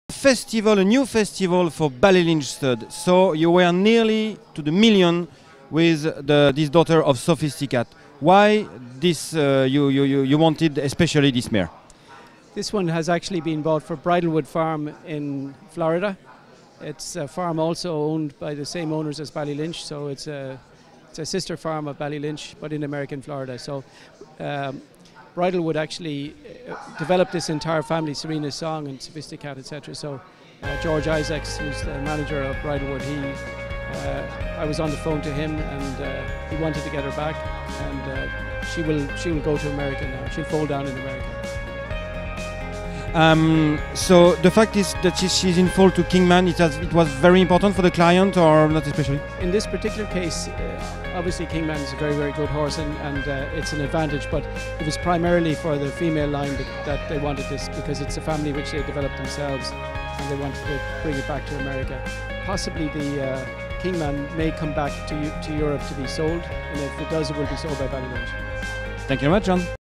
John O'Connor (Ballylinch Stud), acheteur du top price EMBELLISHMENT à 900 000 euros
Agissant pour le compte de l'américain John Malone, propriétaire de Bridlewood Farm en Floride et de Ballylinch Stud en Irlande, John O'Connor a acquis Embellishment, fils de Galileo pleine de Kingman, pour 900 000 euros. C'est le top price de la vente d'élevage de décembre 2015 Arqana à Deauville.